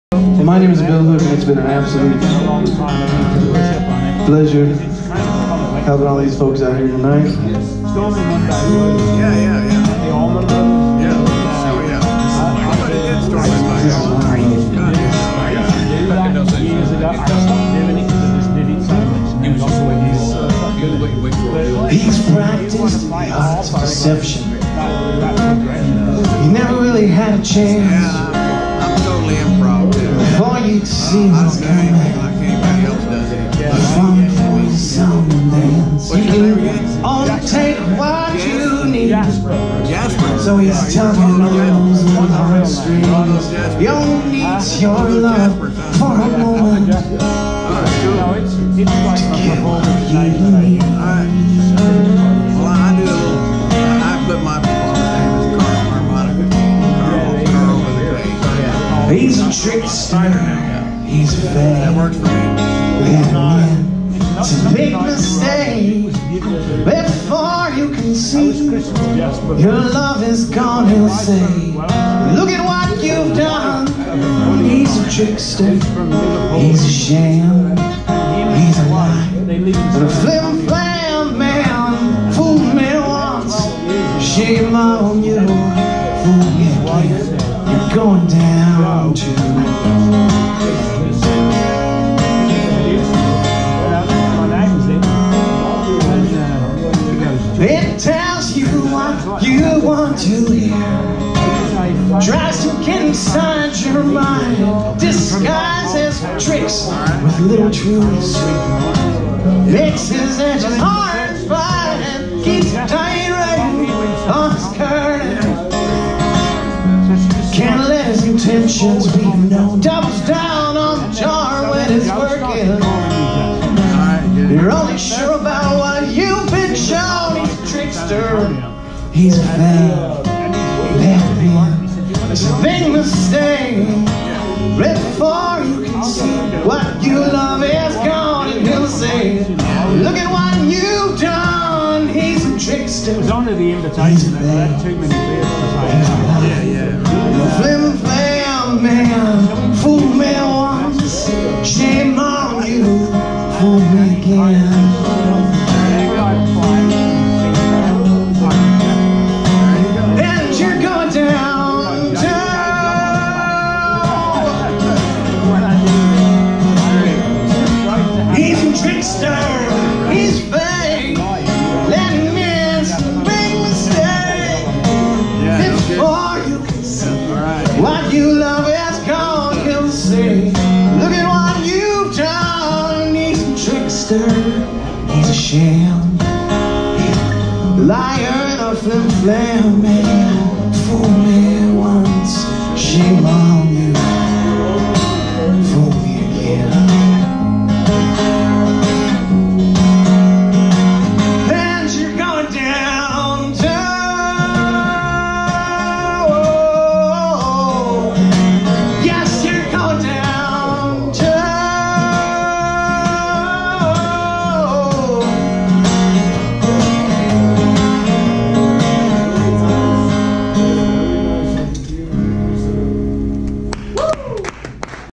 The Trixter May 16, Guitars and Growlers Bill Hook Music
Closing out Songwriter Night at Guitars and Growlers Richardson, TX. May 16, 2018